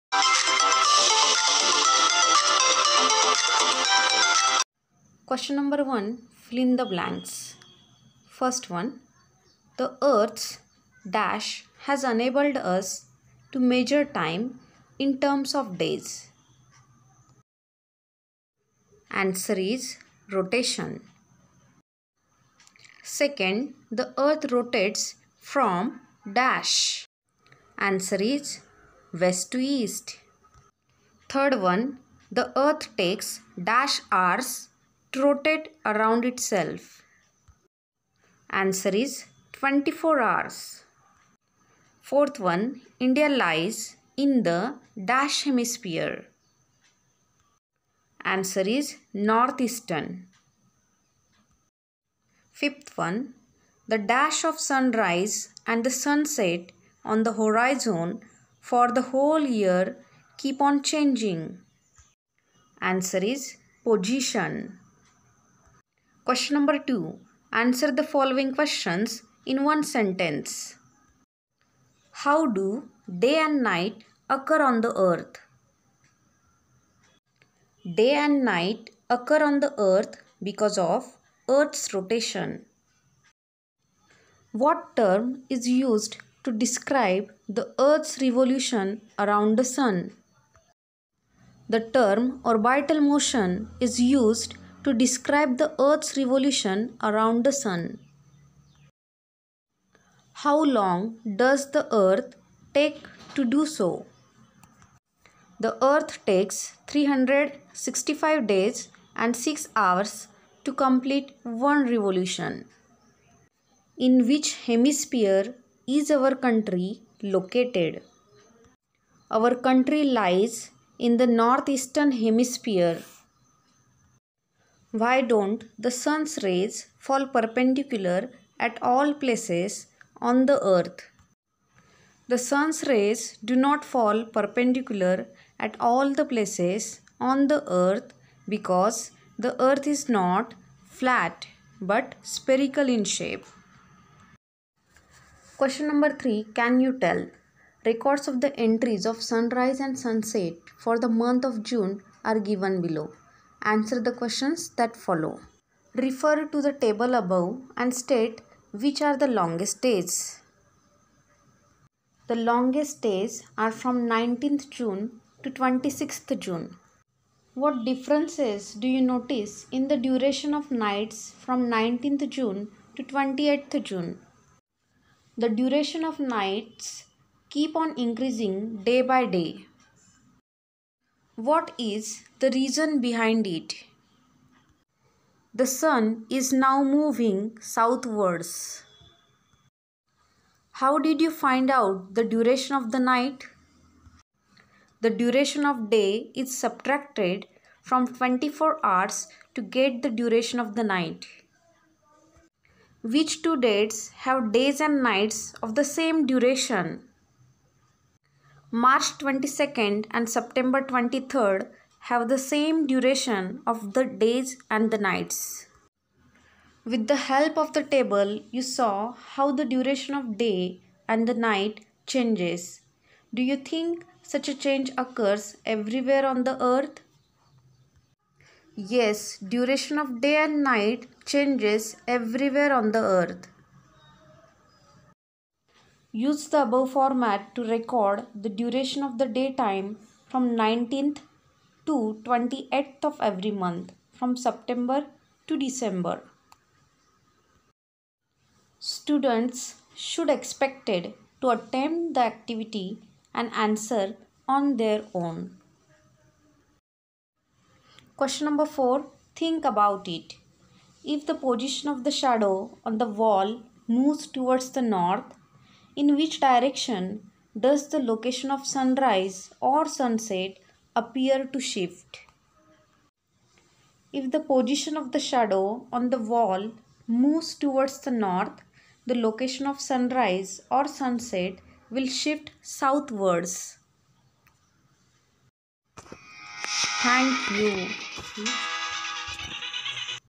Geography/Class 7/Lesson No. 1/How Seasons Occur Part-1/Questions and Answers
Questions and answers based on the lesson

 Based on syllabus of State Board of Maharashtra
Video by Sushama Kanse